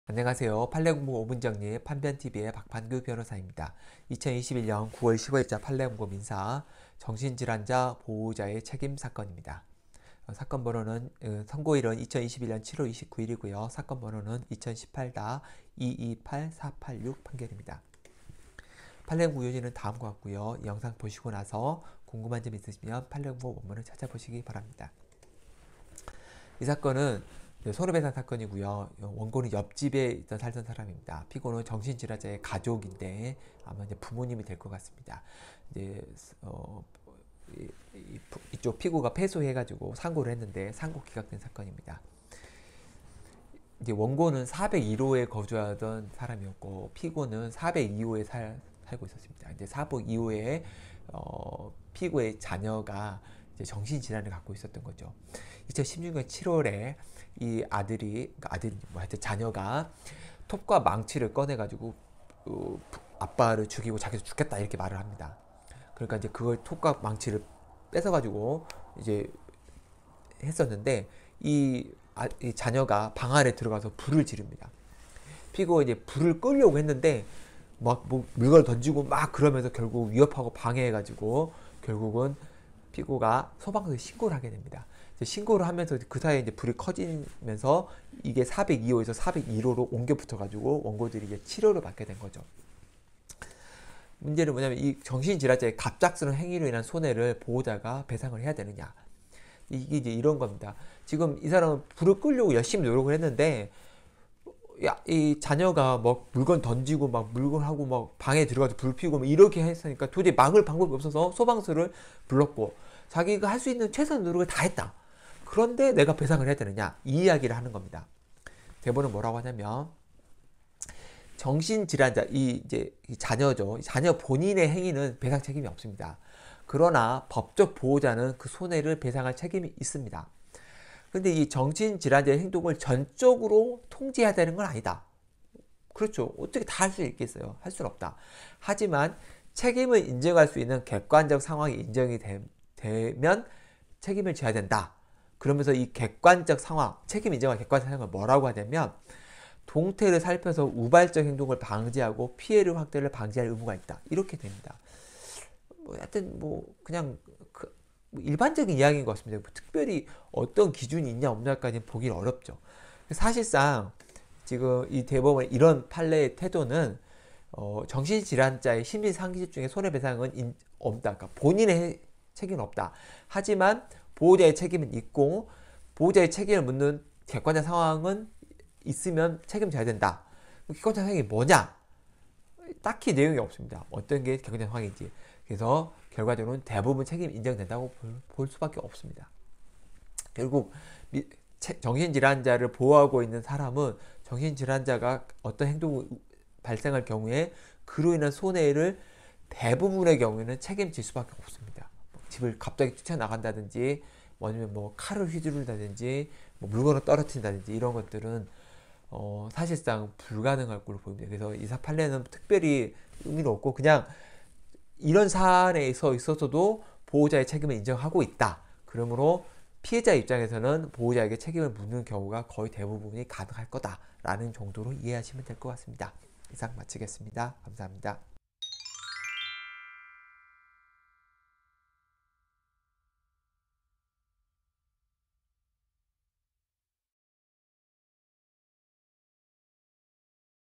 정신질환자 보호자의 책임 사건 [21.9.15.자 판례공보(민사)]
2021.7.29. 선고 2018다228486 판결
손해배상(기)

원고 옆집 입주자
피고 정신질환자의 가족(상고인)

상고기각

원고는 401호 거주자
피고는 402호, 가족 중 정신질환자 A(양극성 정동장애 3급)
16.7.4 A는 톱과 망치 꺼내 피고 죽이고, 자기도 죽겠다고 말함
A는 방 안에 들어가 불을 지름, 피고가 불을 끄려 했으나 A가 위협하고 방해함
피고가 소방서 신고
화재로 401호에 옮겨붙어 원고들이 치료를 받음

정신질환자의 갑작스런 행위로 인한 손해를
보호자가 배상해야 하나?

정신질환자의 심신상실 중 행위는 배상책임이 없음
그러나 그 법적 보호자는 그 손해를 배상할 책임 있음
정신질환자의 행동을 전적으로 통제하여야 하는 것은 아니고
책임 인정할 수 있는 객관적 상황이 인정되어야 함
동태를 살펴 우발적 행동 방지하고, 피해 확대 방지할 의무 있음

실무활용
정신질환자는 심신상실 중 손배책임 없음
보호자가 책임져야 함
책임을 묻는 것이 타당한 객관적 상황
대부분 책임 인정된다고 볼 수 있음